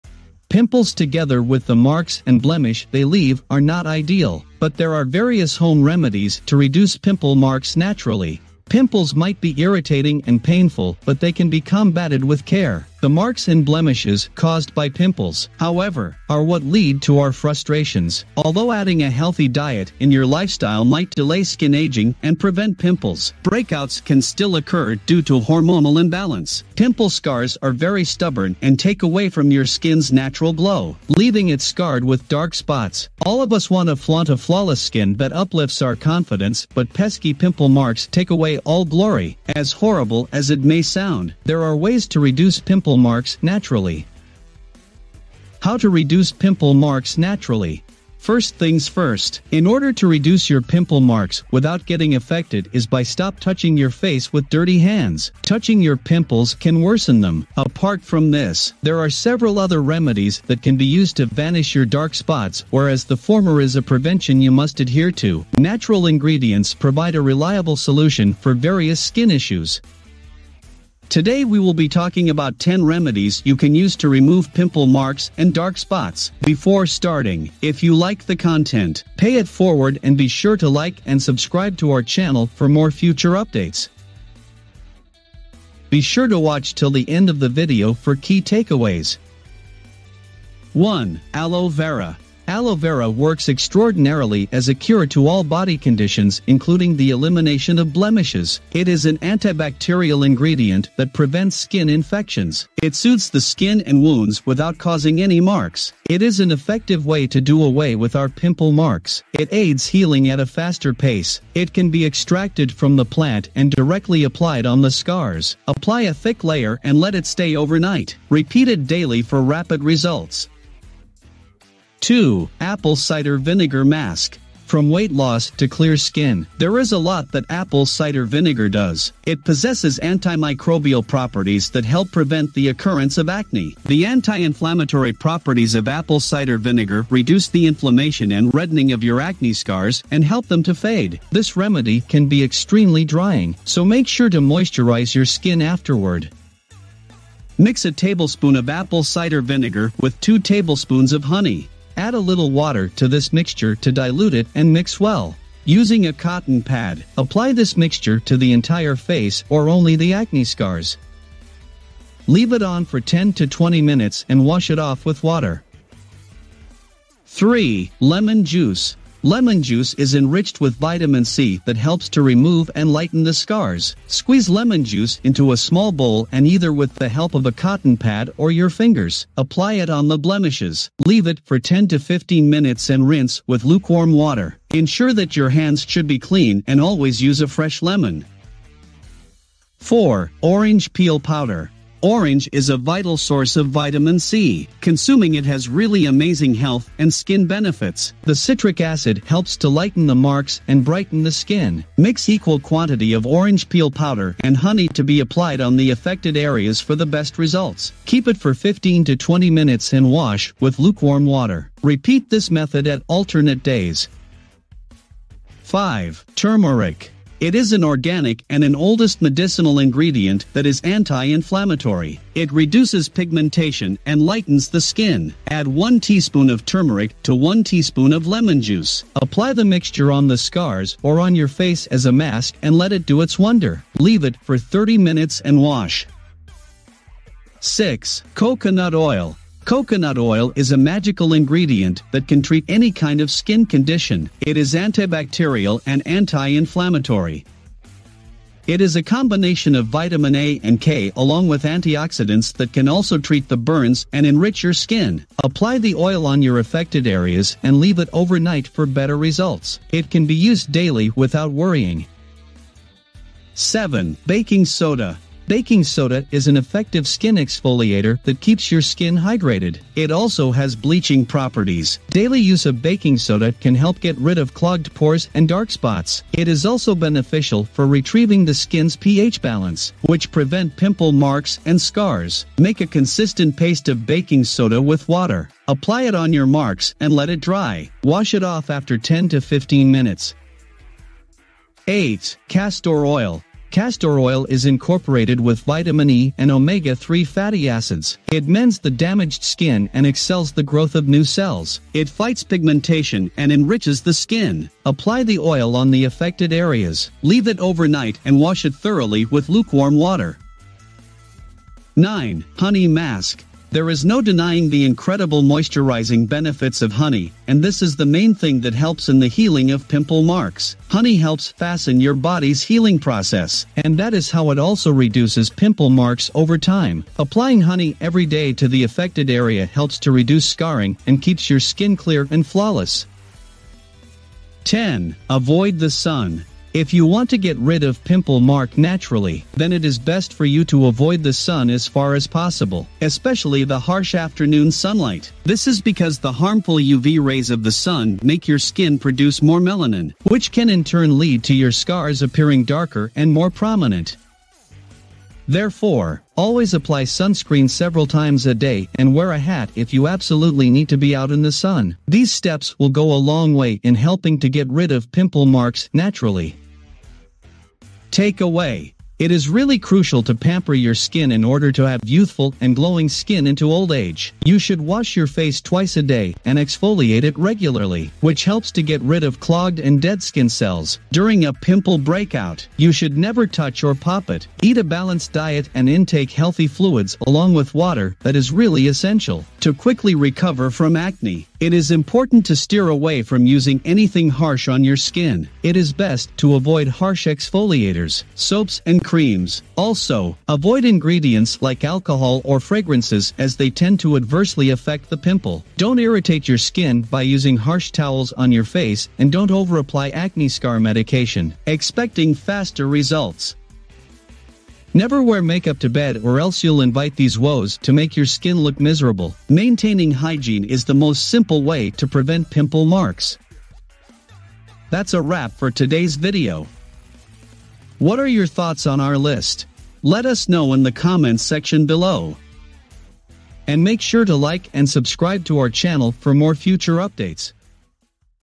How To Remove Pimples and Dark Spots Overnight | Safe Pimple and Acne Treatments (Home Remedies)
Pimples might be irritating and painful but they can be combated with care. The marks and blemishes caused by pimples, however, are what lead to our frustrations. Although adding a healthy diet in your lifestyle might delay skin ageing and prevent pimples, breakouts can still occur due to hormonal imbalance. Pimple scars are very stubborn and take away from your skin's natural glow, leaving it scarred with dark spots. All of us want to flaunt a flawless skin that uplifts our confidence but pesky pimple marks take away all glory. As horrible as it may sound, there are ways to reduce pimple marks naturally.